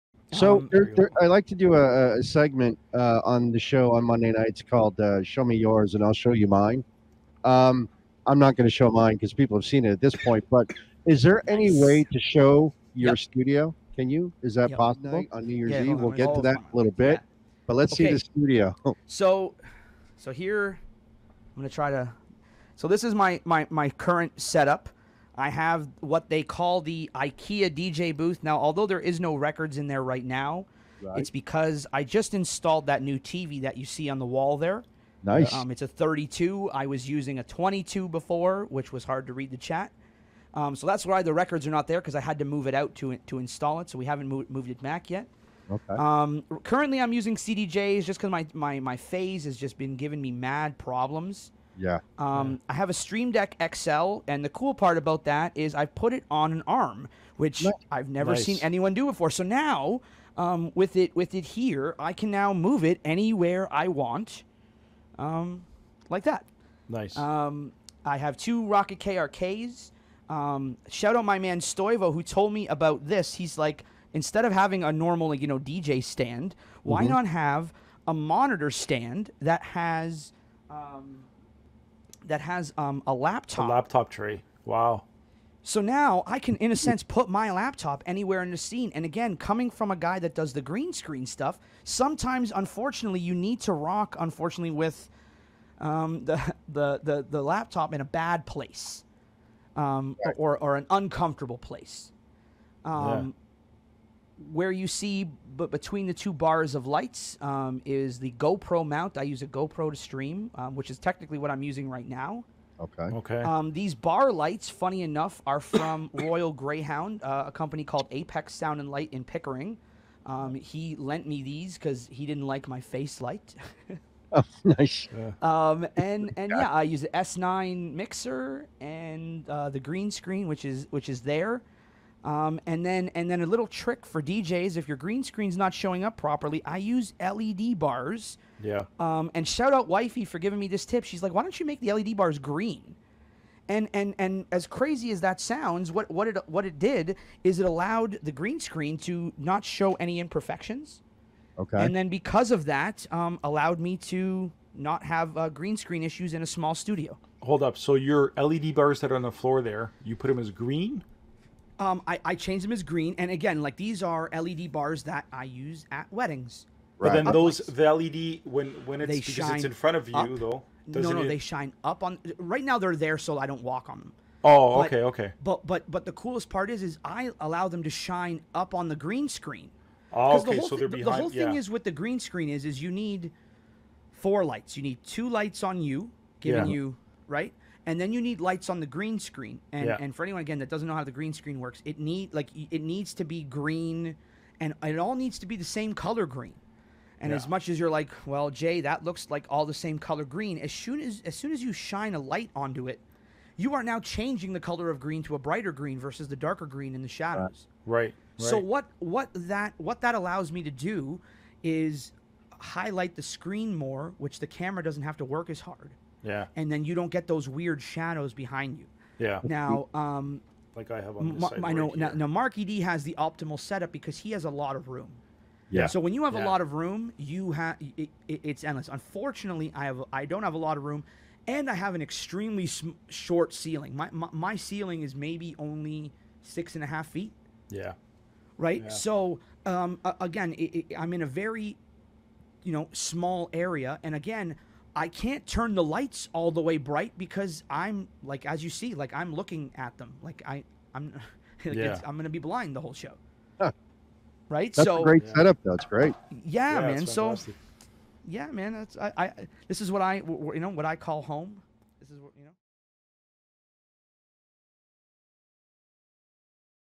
TWITCH DJ - STREAMING SET UP
Hey All,

This clip is from an interview I did with DJ Staples and the roundtable on twitch. I breakdown my current streaming set up and drop a couple gems for all streams in the equipment department to make your set up WAY MORE COMFORTABLE.

Please follow me elsewhere if ya got time!